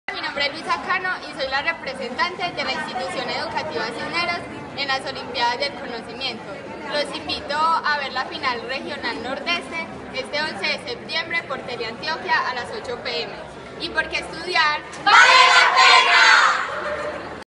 OlimpiadasDelConocimiento | Luisa María Cano Uribe, Cisneros
Esta estudiante de grado once de la Institución Educativa Cisneros, representará a su municipio en la semifinal Nordeste de las Olimpiadas del Conocimiento.